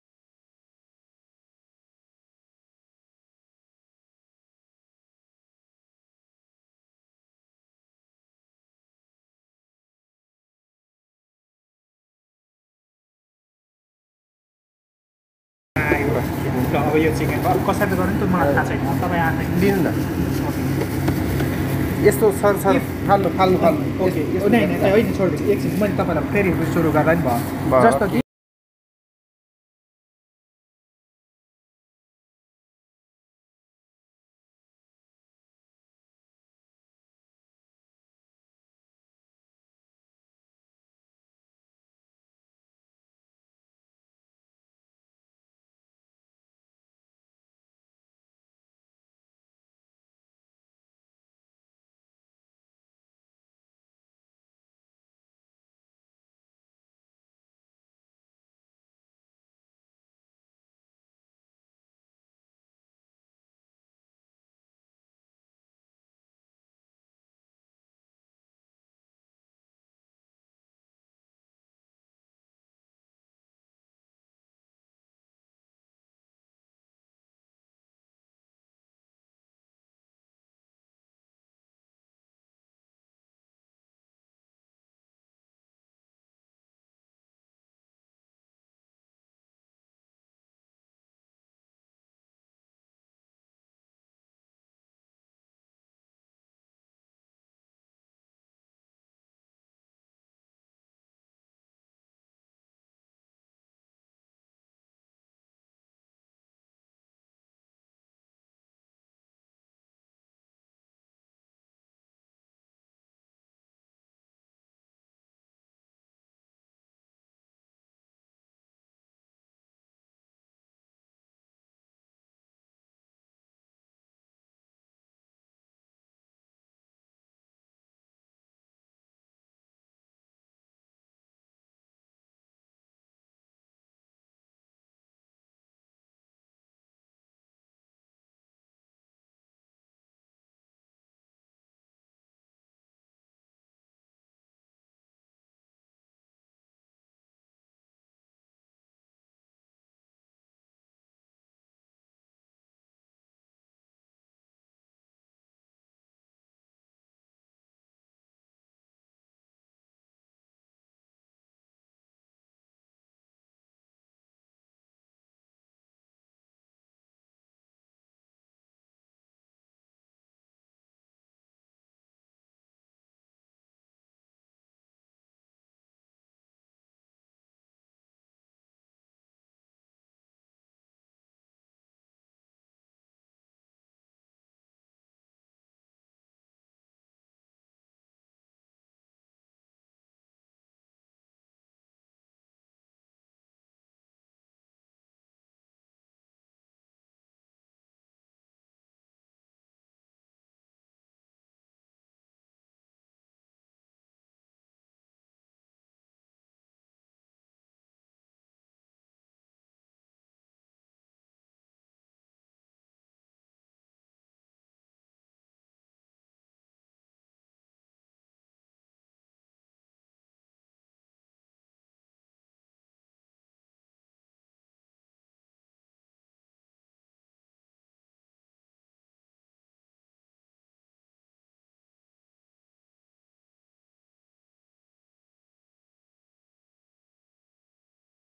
how to make perfect pizza! पिज्जा बनाउने तरकारी
how to make perfect pizza you should check this video and learn more. you can make at home with your partner also and enjoy yammi😊 pizza 😊